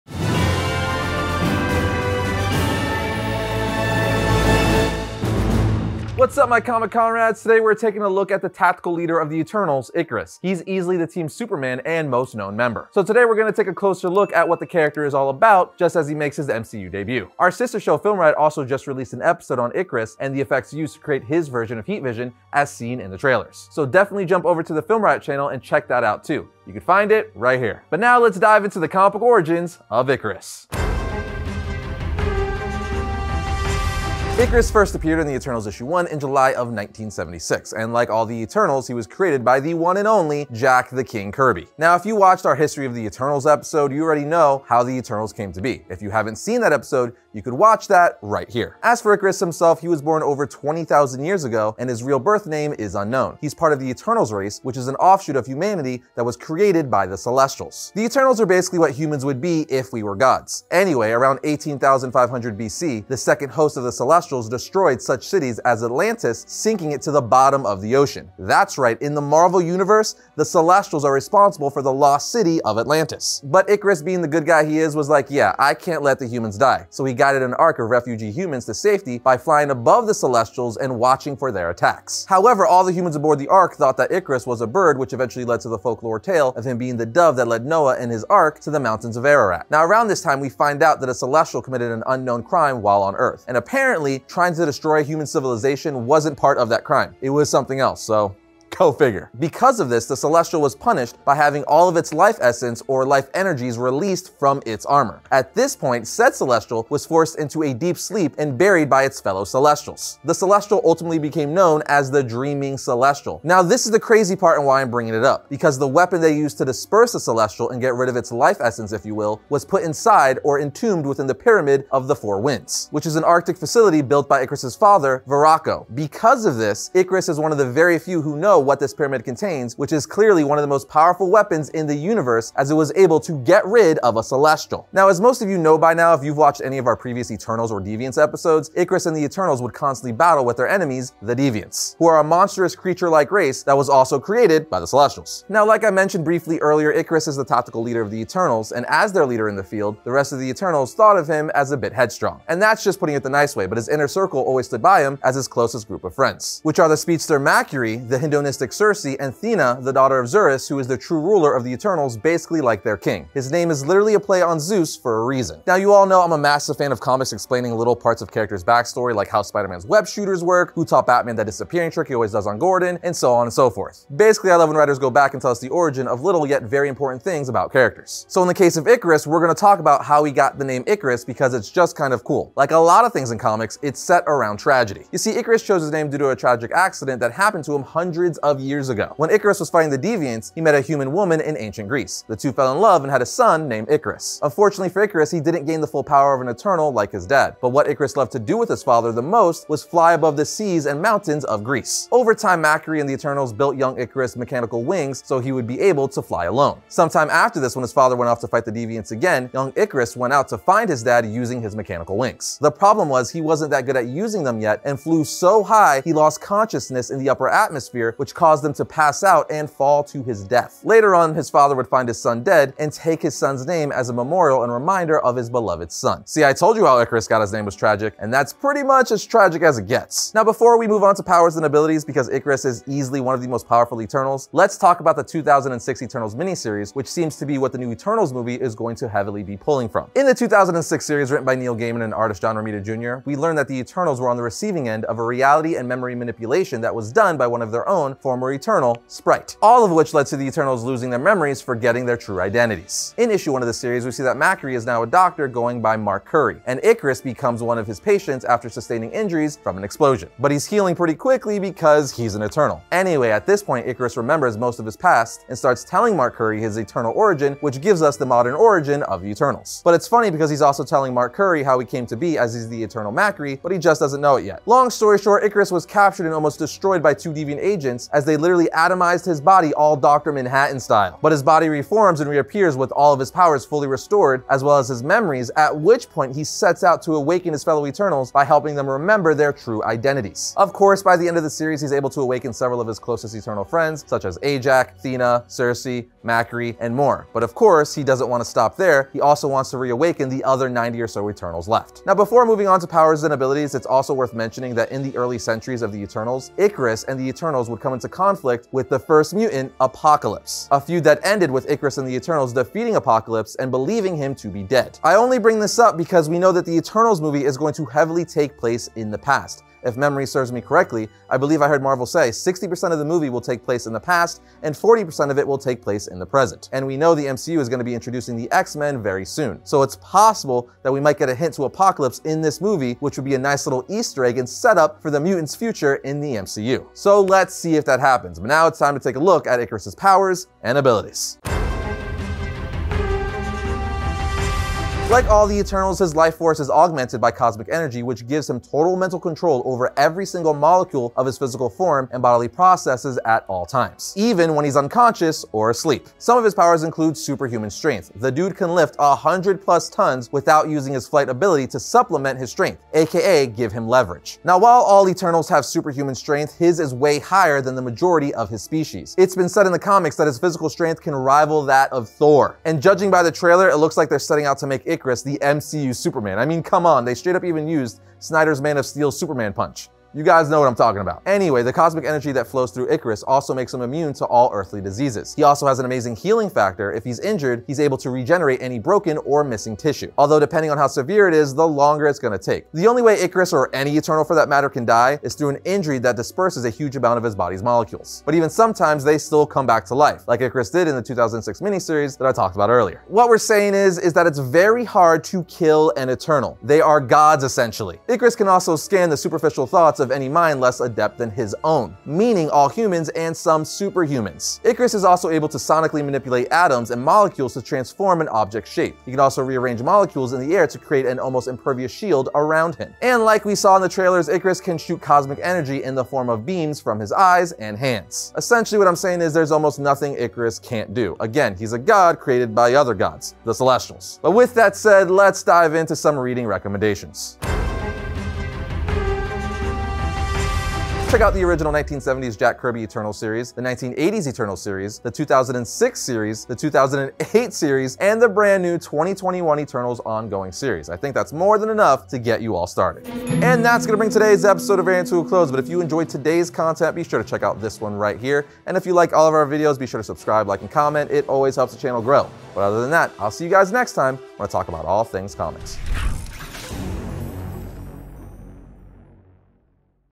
Origin of Ikaris
Today on Variant, we give you the comic book Origin of Ikaris, leader of the Eternals!